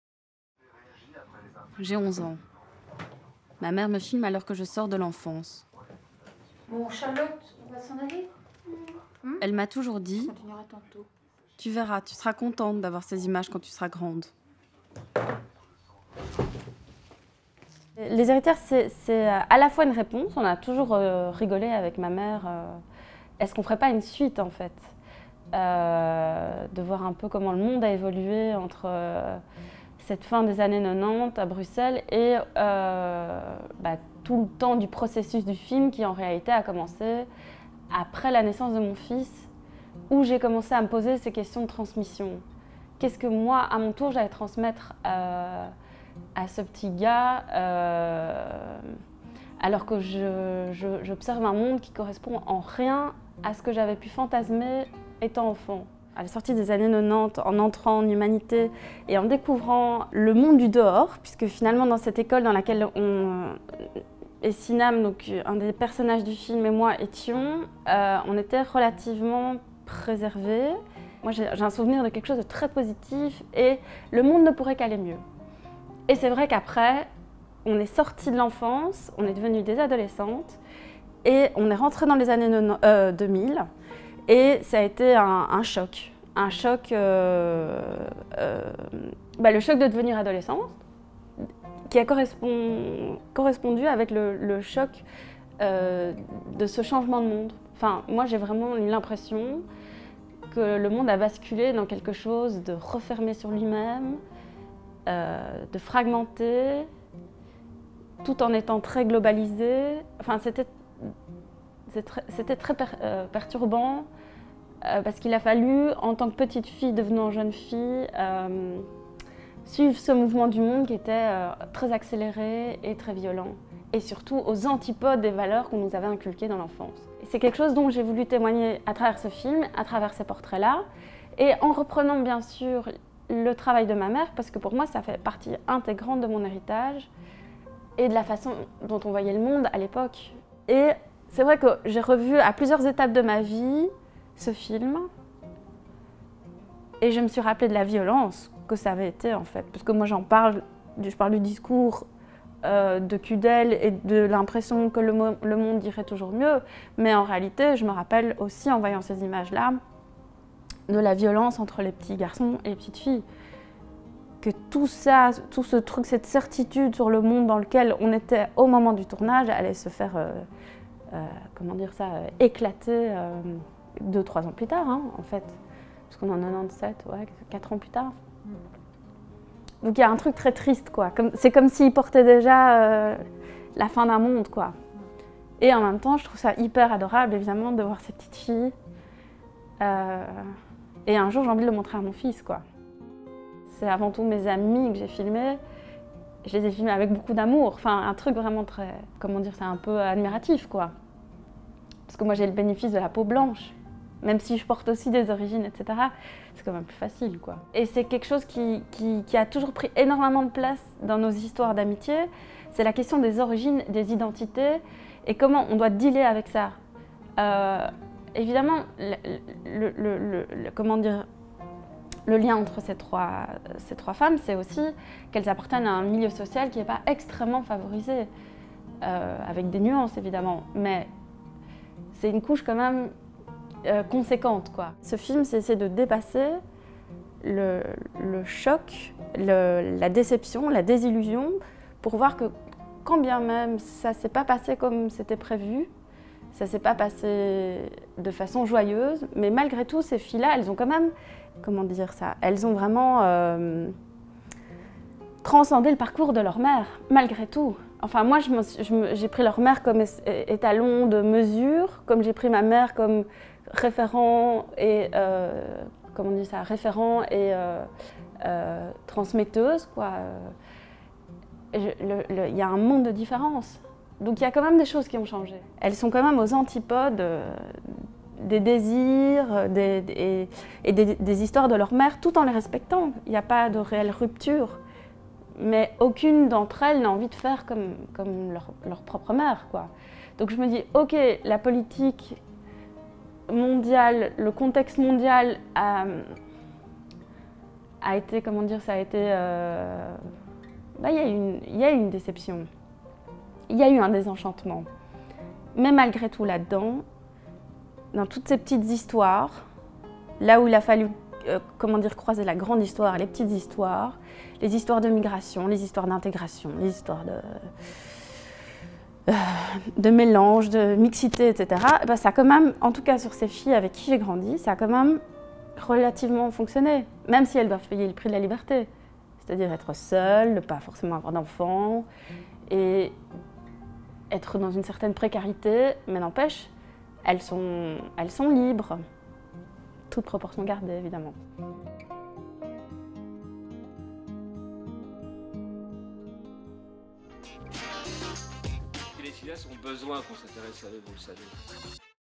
Entretien avec Charlotte Diament
Charlotte Diament arrive avec son premier film, Les Héritières, un moyen-métrage documentaire rétrospectif de Petites filles réalisé en 1997 par Marie-Hélène Massin (sa mère), et produit par Iota Production. Entre le passé et aujourd’hui, Les Héritières interroge l’impact d’une idéologie de l’ouverture promue dans les années 90 qui s’est écroulée une dizaine d’année plus tard. Avec pour toile de fond les petites histoires d’amitié et la grande histoire belge du multiculturalisme, le film entrelace quatre parcours de femmes d’une même génération où chacune questionne son identité dans un contexte politique et social relativement incertain, venant rompre avec un passé qui promettait un monde bien meilleur qu’il ne l’est aujourd’hui.